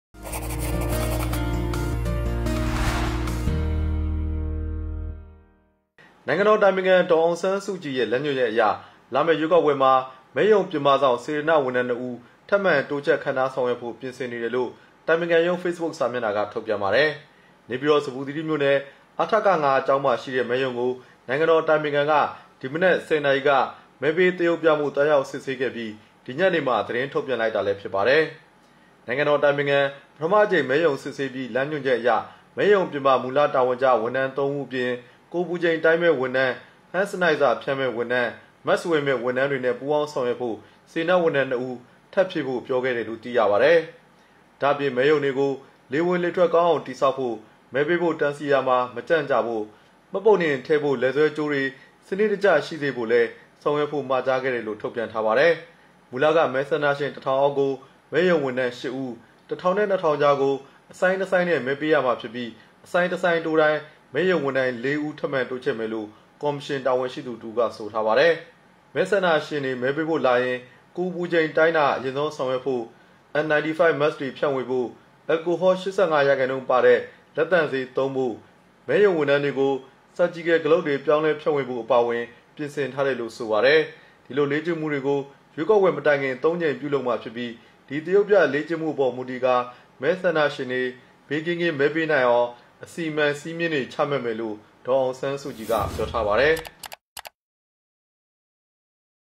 ဒေါ်အောင်ဆန်းစုကြည်လမ်းညွှန်ချက်အရ မဲရုံပြင်ပစေတနာ့နှစ်ဦးထပ်မံတိုးခန့်ထားဆောင်ရွက်ဖို့ ပြင်ဆင်
နိုင်ငံတော်အတိုင်ပင်ခံဒေါ်အောင်ဆန်းစုကြည်ရဲ့ လမ်းညွှန်ချက်အရ လာမယ့်ရွေးကောက်ပွဲမှာ မဲရုံပြင်ပ စောင့် စေတနာ့ဝန်ထမ်းနှစ်ဦးထပ်မံတိုးချဲ့ ခန့်ထားဆောင်ရွက်ဖို့ပြင်ဆင်နေတယ်လို့ အတိုင်ပင်ခံရုံးဖေ့စ်ဘုတ် စာမျက်နှာက ထုတ်ပြန်ပါတယ်။
နေပြည်တော် ဇမူသီရိမြို့နယ် အထက ၅ ကျောင်းမှာရှိတဲ့ မဲရုံကို နိုင်ငံတော်အတိုင်ပင်ခံက ဒီမနက် ၁၀ နာရီက မဲပေးသရုပ်ပြမှုသွားရောက်စစ်ဆေးခဲ့ပြီး ဒီညနေမှာ သတင်းထုတ်ပြန်လိုက်တာလည်း ဖြစ်ပါတယ်။
နိုင်ငံတော်အတိုင်ပင်ခံ ပထမအကြိမ်မဲရုံစစ်ဆေးပြီး လမ်းညွှန်ချက်အရ မဲရုံပြင်ပ မူလတာဝန်ကျ ဝန်ထမ်းသုံးဦး အပြင် ကိုယ်အပူချိန်တိုင်းမယ့်ဝန်ထမ်း၊ Hand Sanitizer ဖြန်းမယ့်ဝန်ထမ်း၊ Mask ဝေမယ့်ဝန်ထမ်းတွေနဲ့ ပူးပေါင်းဆောင်ရွက်ဖို့ စေတနာ့ဝန်ထမ်းနှစ်ဦးထပ်ဖြည့်ဖို့ ပြောခဲ့တယ်လို့ သိရပါတယ်။
ဒါ့အပြင် မဲရုံတွေကို လေဝင်လေထွက်ကောင်းအောင်တည်ဆောက်ဖို့၊ မဲပေးဖို့ တန်းစီရာမှာမကြန့်ကြာဖို့၊ မှတ်ပုံတင်ထည့်ဖို့ လည်ဆွဲကြိုးတွေစနစ်တကျရှိစေဖို့လည်းဆောင်ရွက်ဖို့မှာကြားခဲ့တယ်လို့ ထုတ်ပြန်ထားပါတယ်။
မူလက မဲဆန္ဒရှင် တစ်ထောင်အောက်ကို မဲရုံဝန်ထမ်း ၈ ဦး၊ တစ်ထောင်နဲ့ နှစ်ထောင်ကြားကို အဆိုင်းနှစ်ဆိုင်း နဲ့မဲပေးရမှာဖြစ်ပြီး အဆိုင်းတစ်ဆိိုင်းတိုးတိုင်း မဲရုံဝန်ထမ်း ၄ ဦးထပ်မံတိုးမယ်လို့ ကော်မရှင်တာဝန်ရှိသူတစ်ဦးက ဆိုထားပါတယ်။
မဲဆန္ဒရှင်တွေ မဲပေးဖို့လာရင် ကိုယ်အပူချိန်တိုင်းတာအရင်ဆုံးဆောင်ရွက်ဖို့၊ N95 Mask တွေဖြန့်ဝေဖို့၊အယ်လ်ကိုဟော ၈၅ ရာခိုင်နှုန်းပါတဲ့ လက်သန့်ဆေးသုံးဖို့၊မဲရုံဝန်ထမ်းတွေကို Surgical Glove တွေပြောင်းလဲဖြန့်ဝေဖို့အပါအဝင် ပြင်ဆင်ထားတယ်လို့ ဆိုပါတယ်။
ဒီလို လေ့ကျင့်မှုကို ရွေးကောက်ပွဲ မတိုင်ခင် သုံးကြိမ်ပြု လုပ်မှာဖြစ်ပြီး  ဒီသရုပ်ပြလေ့ကျင့်မှု အပေါ်မူတည်ကာ မဲဆန္ဒရှင်တွေ ဘေးကင်းကင်းမဲပေးနိုင်အောင် အစီအမံစည်းမျည်းတွေ ချမှတ်မယ်လို့ ဒေါ်အောင်ဆန်းစုကြည်က ပြောထားပါတယ်။